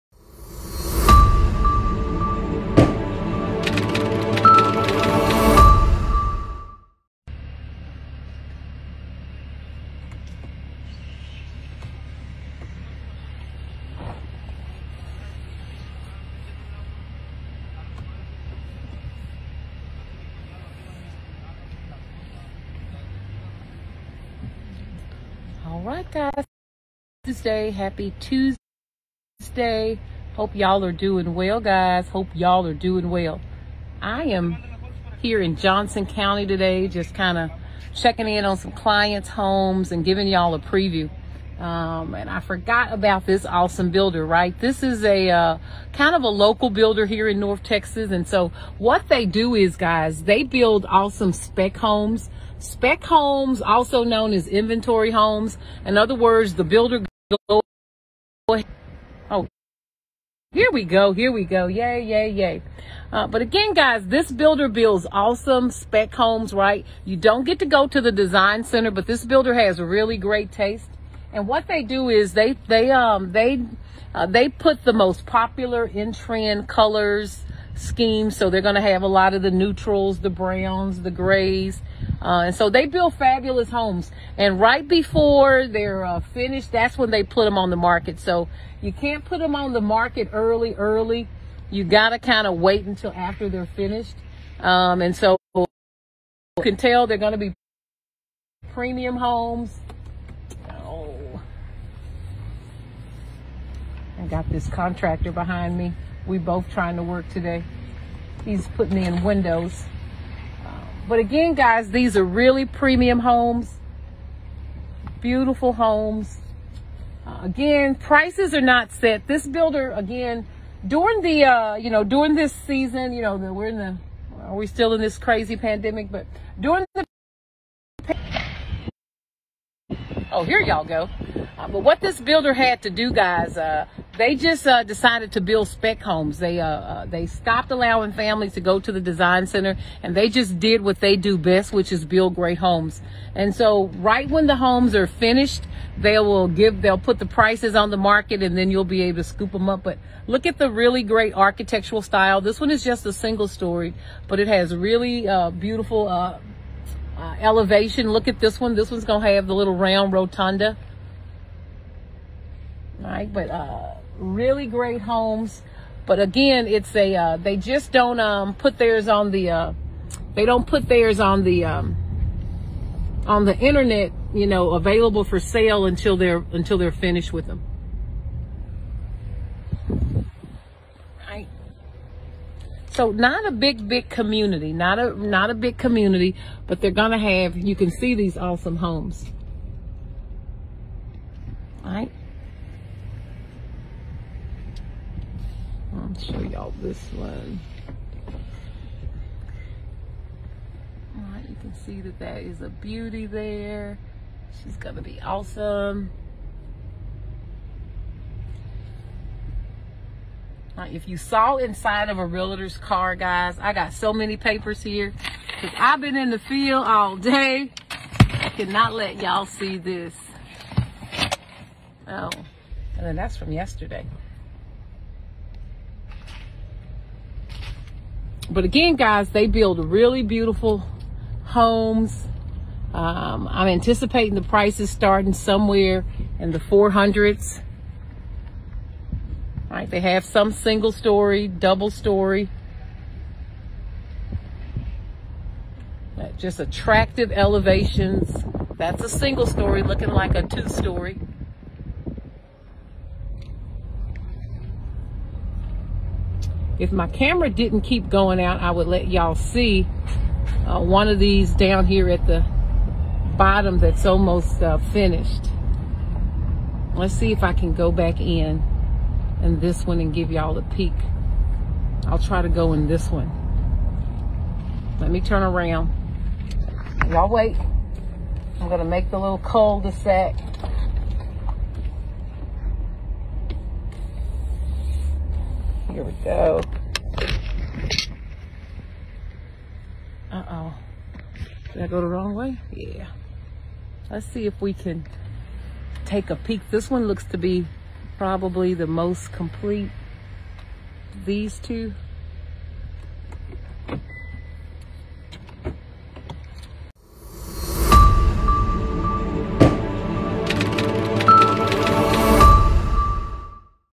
Y’all join Paula as she previews this sweet community in Johnson County with Burleson ISD…
Y’all join me as I preview this sweet community in Johnson County with Burleson ISD…

This builder is building inventory or spec homes and they will be coming to the market soon…

Prices are not set but projected to start around the 400s….

Curious about new construction and not sure where to start?

Sign up on the link for a free consultation🤗

Paula Sanders
Texas Realtor
Brokered by EXP Realty LLC